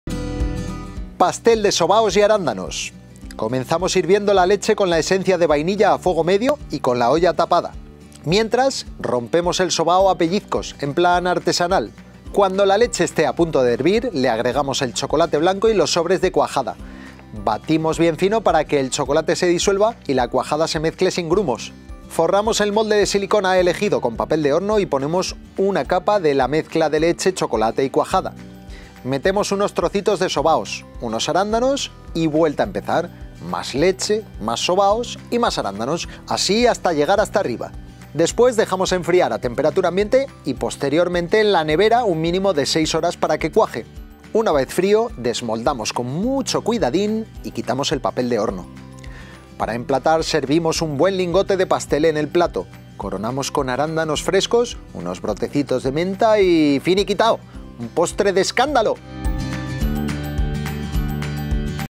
PASTEL DE SOBAOS, ARÁNDANOS Y VAINILLA - LPL PGM 2027 RECETA 02
Ingredientes para 4 personas:
1 l de leche fresca entera, 3 sobres de cuajada, 1 tapón de esencia de vainilla, 100 g de chocolate blanco, 150 g de sobaos pasiegos y 100 g de arándanos.
Además, menta fresca.

Elaboración:
Comenzamos hirviendo la leche con la esencia de vainilla, a fuego medio, con la olla tapada.
Mientras, rompemos el sobao a pellizcos, en plan artesanal.
Cuando la leche esté a punto de hervir, le agregamos el chocolate blanco y los sobres de cuajada. Batimos bien fino, para que el chocolate se disuelva y la cuajada se mezcle sin grumos.
Forramos el molde de silicona elegido con papel de horno y ponemos una capa de leche, choco y cuajada. Metemos unos trocitos de sobao, unos arándanos y vertemos más mezcla de leche. Repetimos: sobaos, arándanos y leche, y así hasta llegar hasta el final. Luego lo dejamos templar y después lo reposamos en la nevera un mínimo de 6 horas, para que cuaje.
Una vez frío, desmoldamos y quitamos el papel de horno.
Para emplatar, servimos un buen lingote de pastel en el plato. Coronamos con arándanos frescos, unos brotes de menta fresca y finiquitao. Un postre de escándalo.